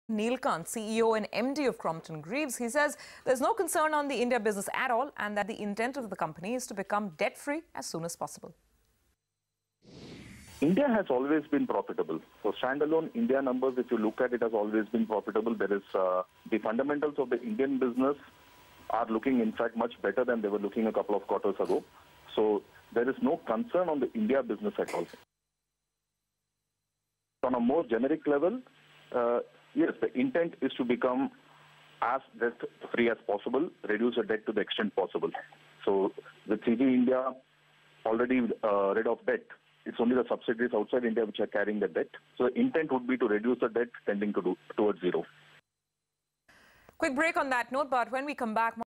Crompton Greaves Sells International T&D Business
Crompton Greaves has sold its international T&D business. KN Neelkant, CEO and MD of Crompton Greaves states that there is no concern on the India business at all and that the intent of the company is to become as debt free as soon as possible.